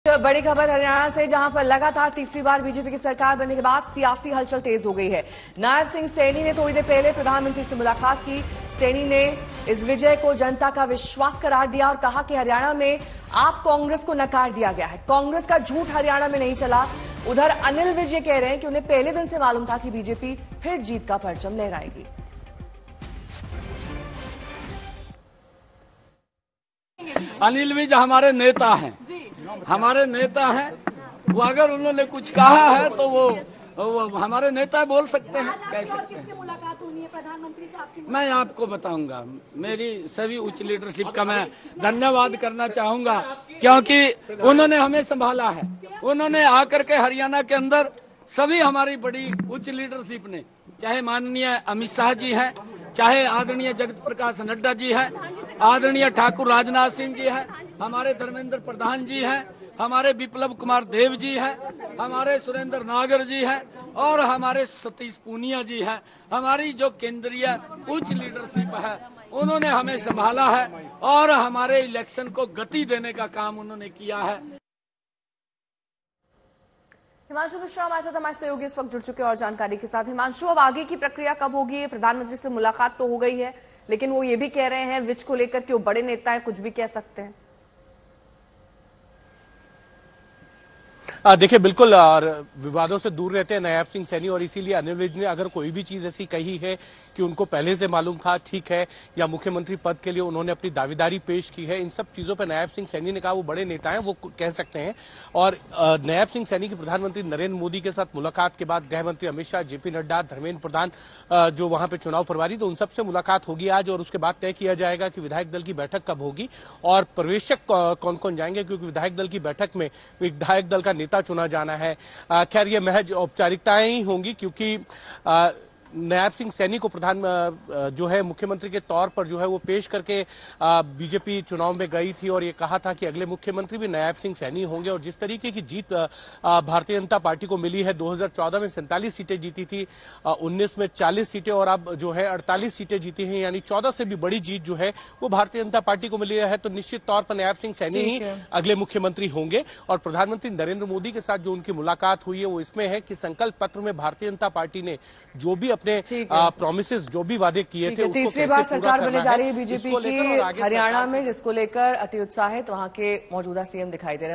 Haryana BJP Win: Nayab Singh Saini से मिले PM Modi, Congress पर बरसे नायब सिंह सैनी | Aaj Tak
हरियाणा में लगातार तीसरी बार बीजेपी सरकार के बाद सियासी हलचल तेज है . नायब सिंह सैनी ने थोड़ी देर पहले पीएम मोदी से मुलाकात की सैनी ने विजय को जनता का विश्वास करार दिया और कहा हरियाणा नें आप- कांग्रेस को नकार दिया.. कांग्रेस का झूठ हरिय़ाणा में नहीं चला- उधर अनिल विज कह रहे हैं उन्हें पहले दिन से मालूम था कि बीजेपी फिर जीत का परचम लहराएगी ।